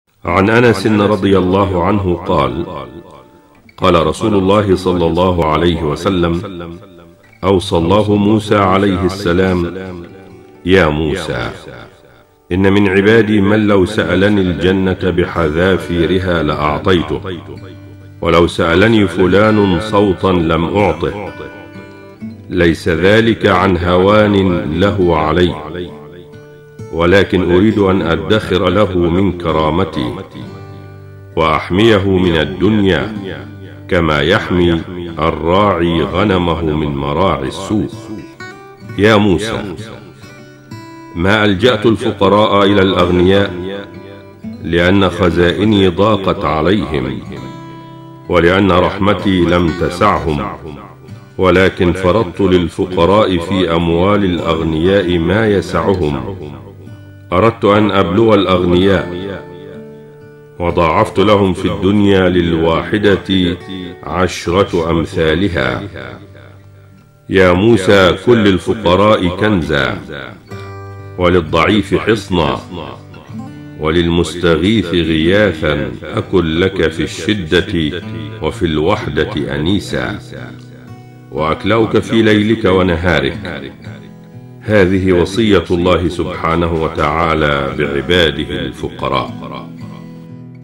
يا موسى ما ألجاتُ الفقراء إلى الأغنياء
فعن أنس بن مالك – رضي الله عنه – في الحديث الذى أخرجه البخاري قال: سمعتُ رسول الله – صلى الله عليه وسلم – يقول: أوحى الله إلى موسى (يا موسى إن من عبادي لو سألني الجنة بحذافيرها لأعطيتُه إياها ، ولو سألني جراب سوطاً من الدنيا لم أُعطيه إياه ، وليس ذلك لهوان علي – ولكن لما أحبُ لهُ أن أدخره له من كرامتى وأحميه من الدنيا كما يحمي الراعي غنمه من مراعي السوء).

يا موسى ما ألجاتُ الفقراء إلى الأغنياء لأن خزائني ضاقت بهم أو لأن رحمتي لم تسعهم لا ، ولكن لأني فرضت على الأغنياء للفقراء ما يسعهم ، وبلوت الأغنياء بالفقراء ، فإن هم أكرموهم وإن هم أعطوهم أتممت عليهم نعمتي وضاعفت لهم الحسنة الواحدة بعشر أمثالها ، (يا موسى كن للفقير حصناً وكن للضعيف كنزاً وكن للمستجير غيثاً ، أكن لك في الشدة صاحباً وفي الوحدة أنيساً). وصدق رسول الله – صلى الله عليه وسلم.